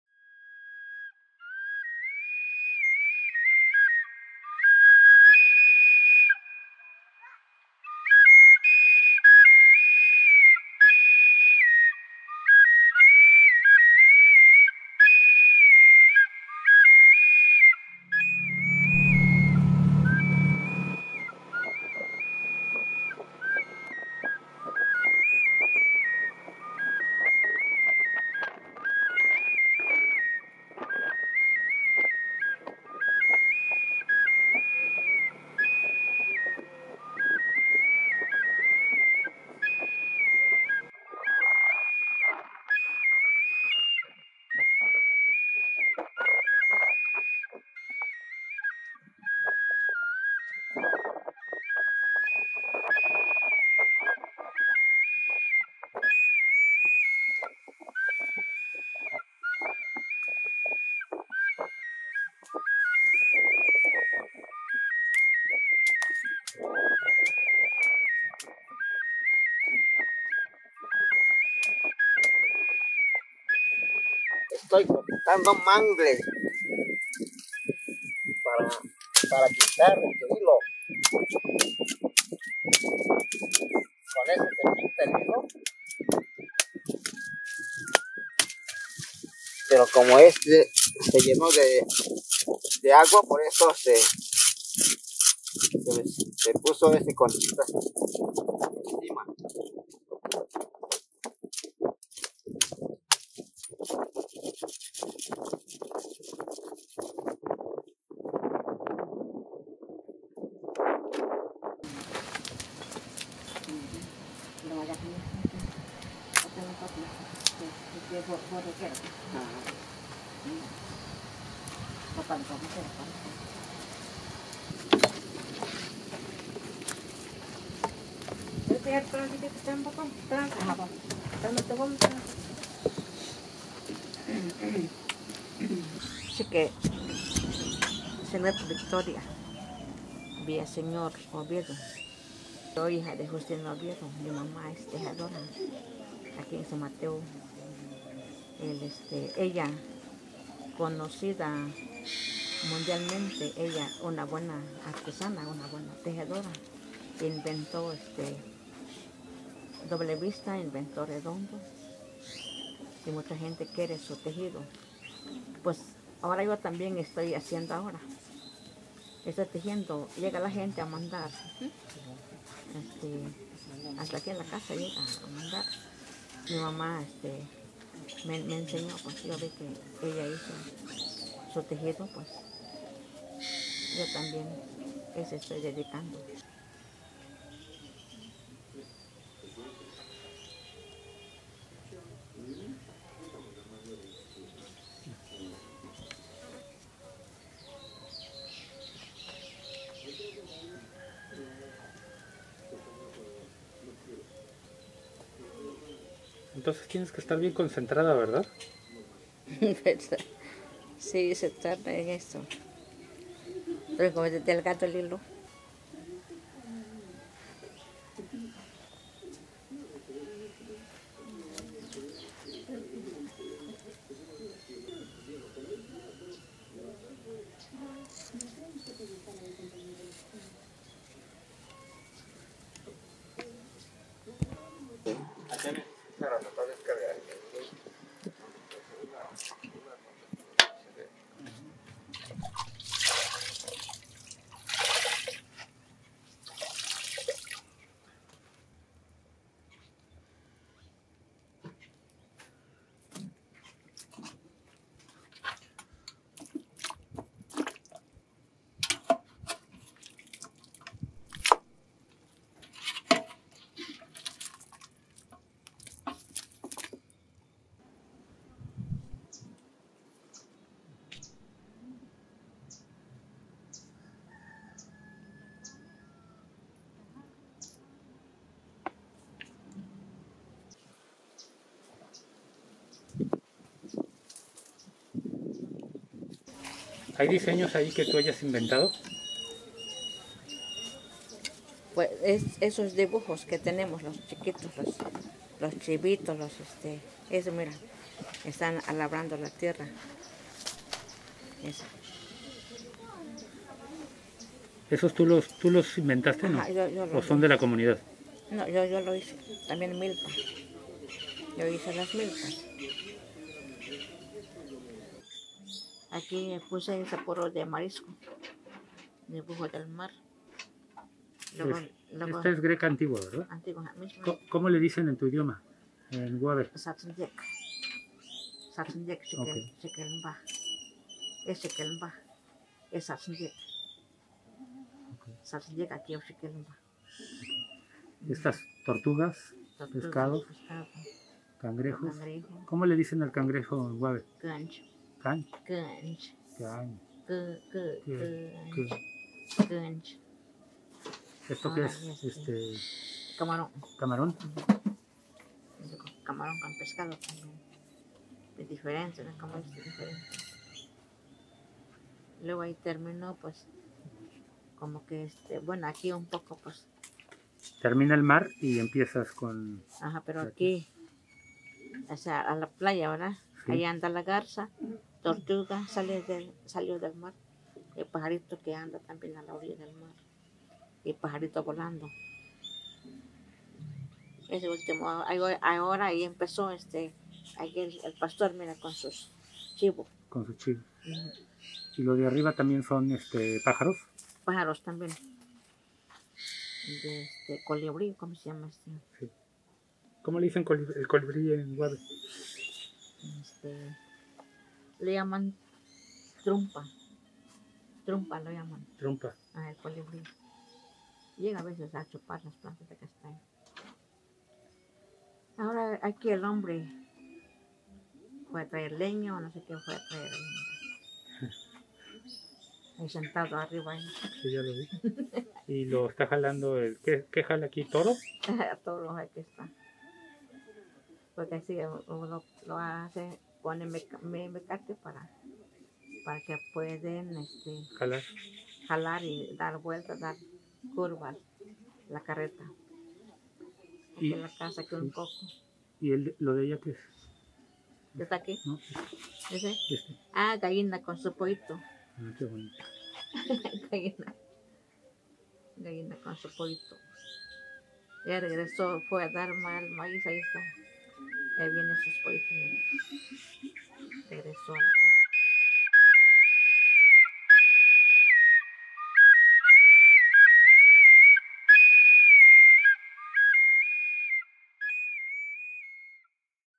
Telar de mangle rojo (español)
Este video es parte de la exposición "Los Colores de la Tierra", que presenta los métodos de creación del telar teñido con mangle rojo.